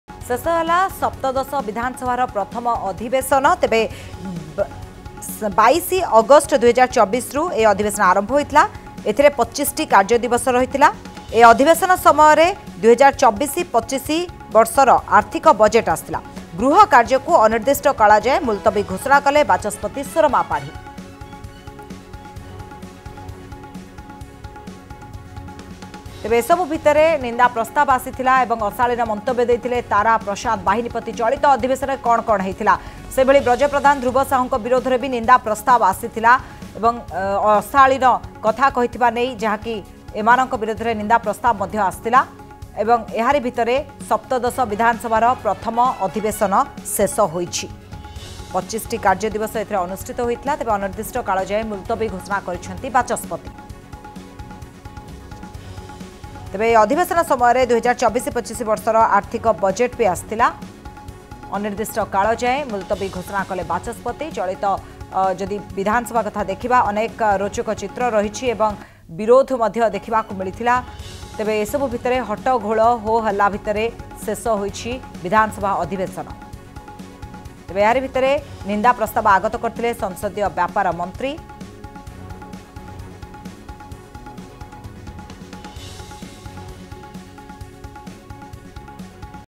First Session Of 17th Odisha Assembly Concludes
The first session of 17th Odisha Legislative Assembly was adjourned two days ahead of the schedule on Wednesday.